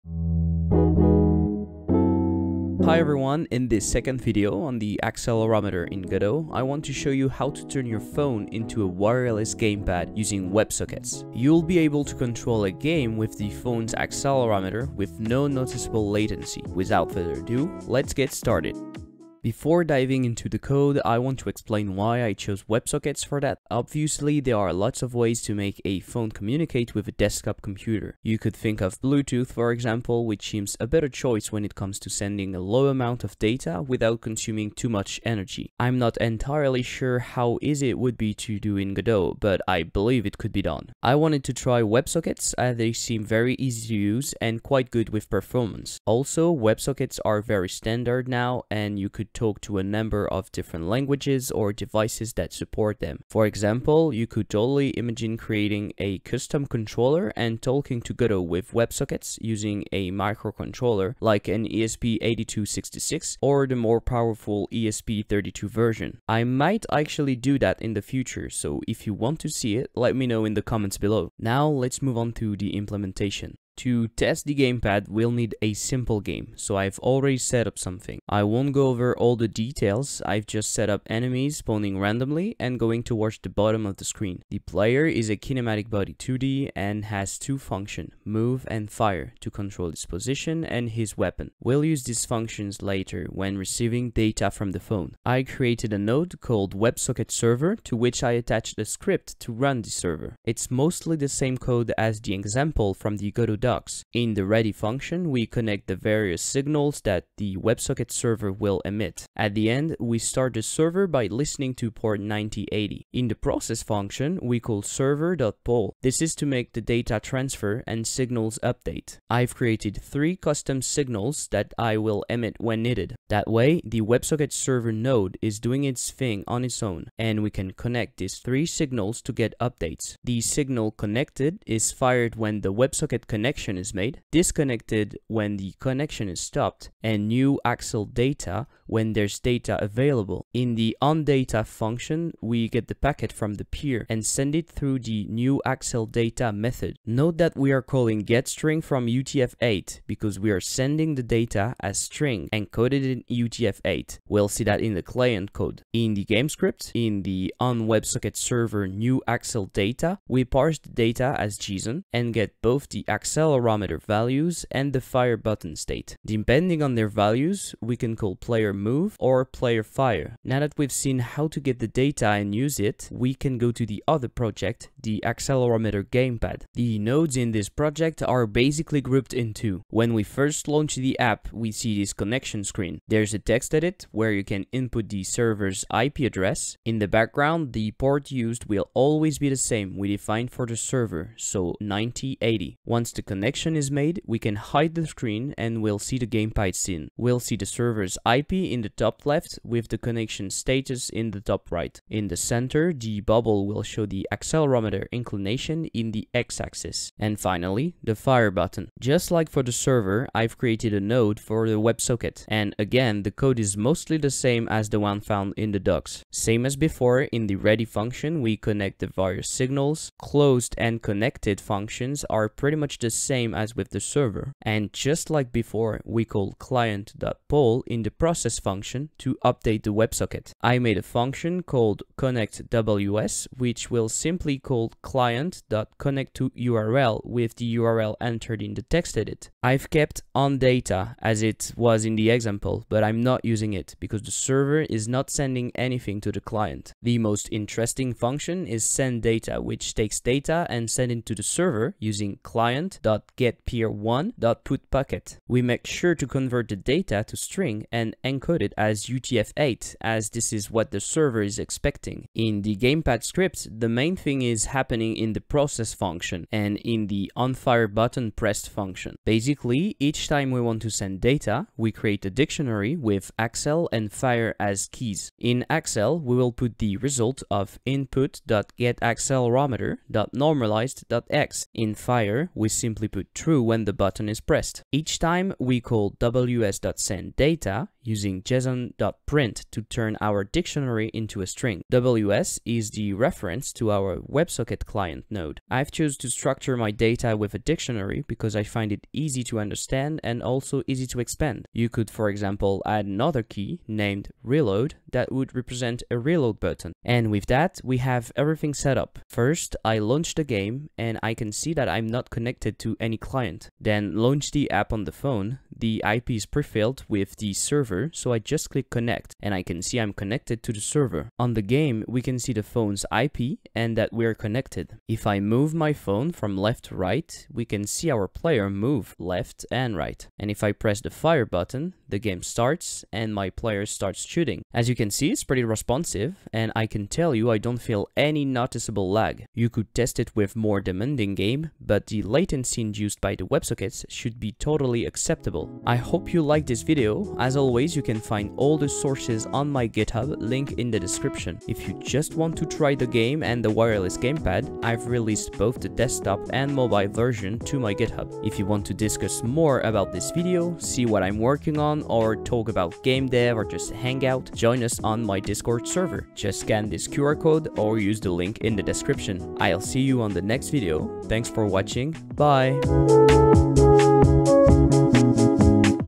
Wireless gamepad using websockets & accelerometer in GODOT
In this second part on the accelerometer in Godot, I'm showing you how to turn your phone into a wireless gamepad, using websockets!

CHAPTERS
00:00 - Intro with result
00:19 - Why websockets?
01:12 - Game scene
01:40 - Websocket server
02:37 - Game script
02:55 - Gamepad scene
03:29 - Websocket client
04:23 - Gamepad script
05:09 - Result
05:45 - Outro